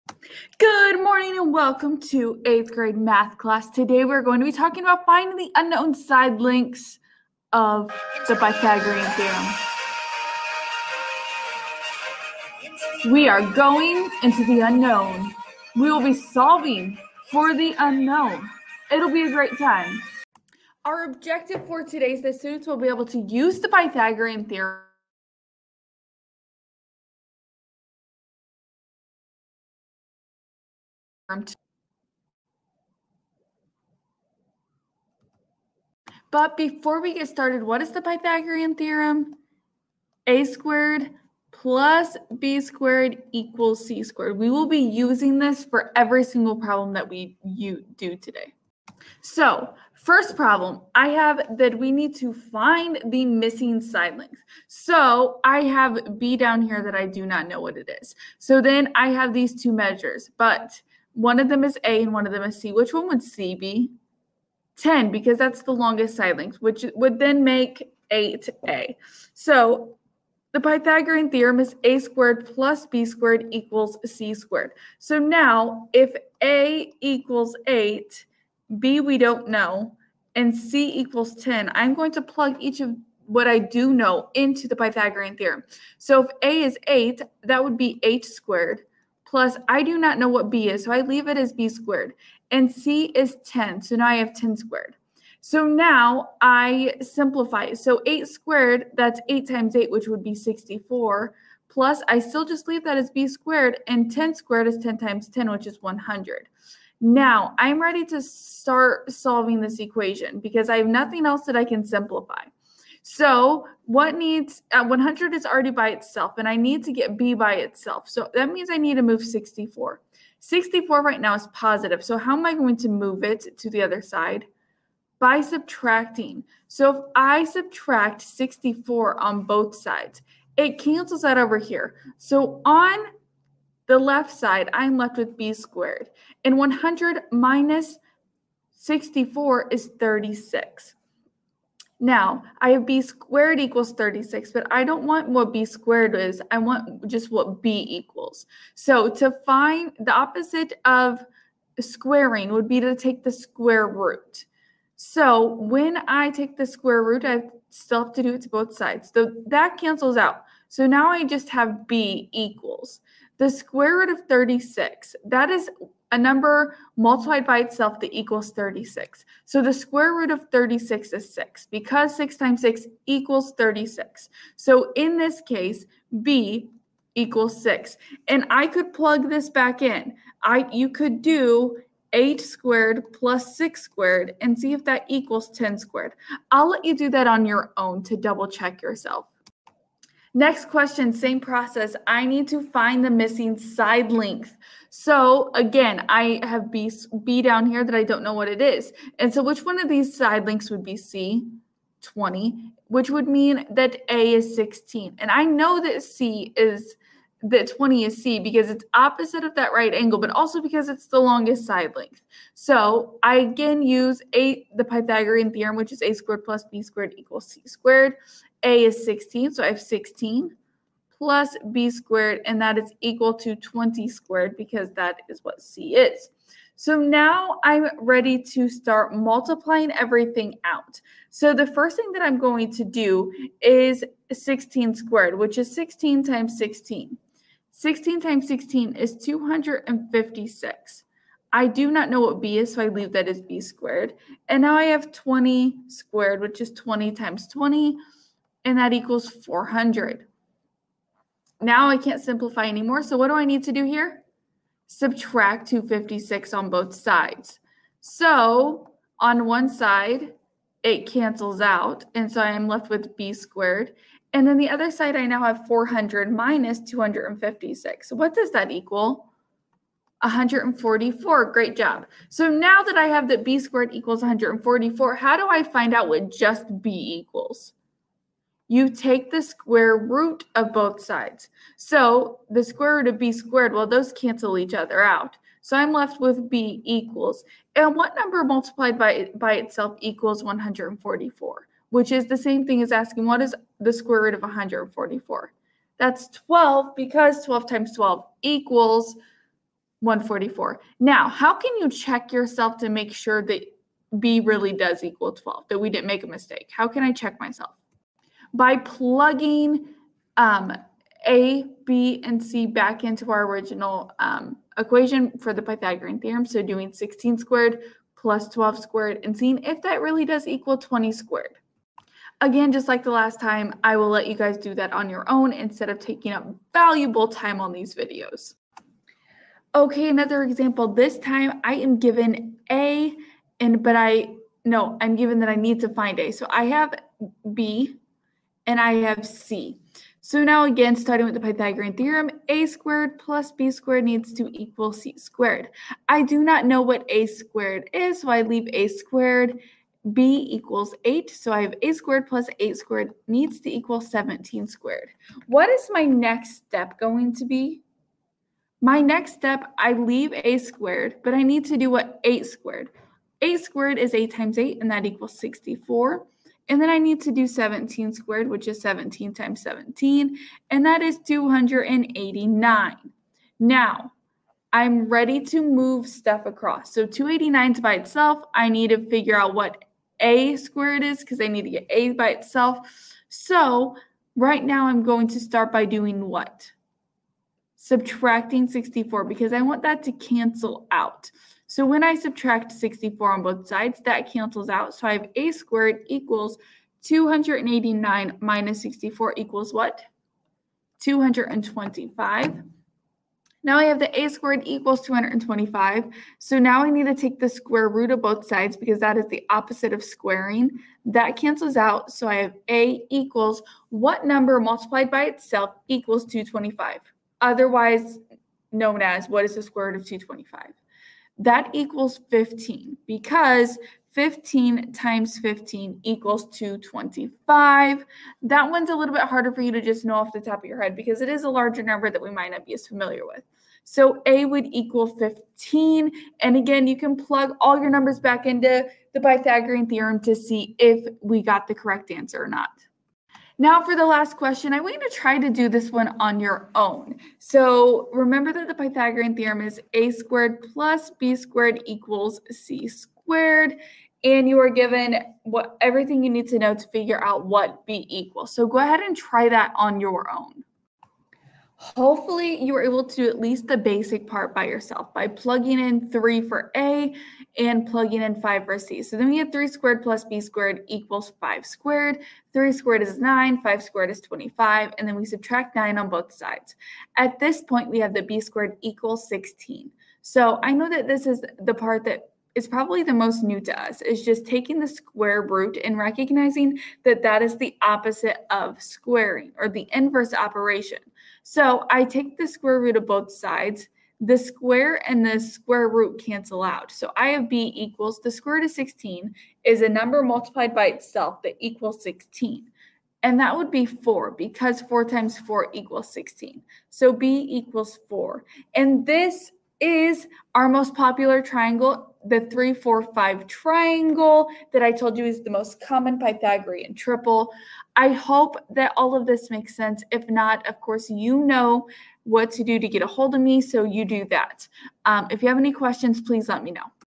Pythagorean Theorem: Solving for an Unknown Side
In today's video lesson we will learn how to solve for an unknown side length of a right triangle by using the Pythagorean Theorem.